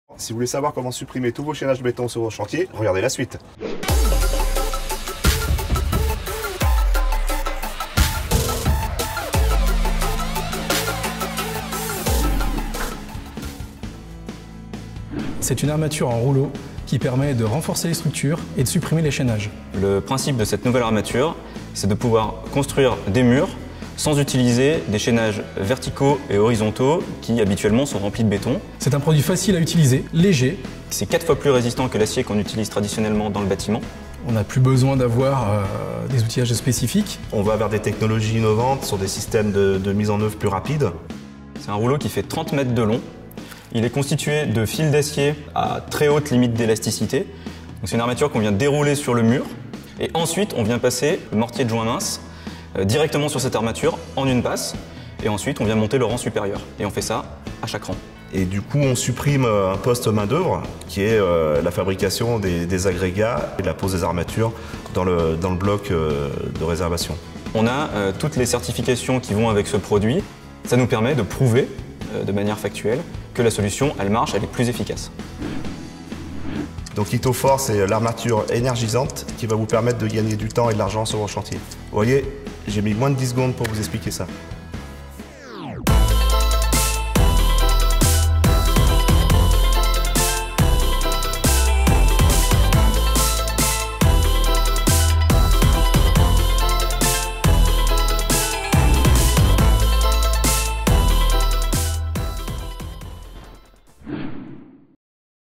XELLA - YTOFOR, l'armature énergisante qui booste les chantiers
XELLA innove et lance sa nouvelle armature métallique :YTOFOR® est une armature conçue pour
renforcer les murs maçonnés et leur conférer un
comportement mécanique supérieur.

Composé d’une maille de 14 fils d’acier tressés avec de la fibre de verre, YTOFOR® s’intègre dans
le joint de colle entre les blocs.
YTOFOR® permet ainsi de supprimer les chaînages en béton armé traditionnels ou de les compléter
lorsque leur utilisation est obligatoire.
A la fois résistante et souple, YTOFOR® se présente sous forme de rouleau, facile à stocker, à
transporter, à dérouler et à découper.

🎙️Des podcasts à écouter et réécouter sur vos plateformes d’écoute préférées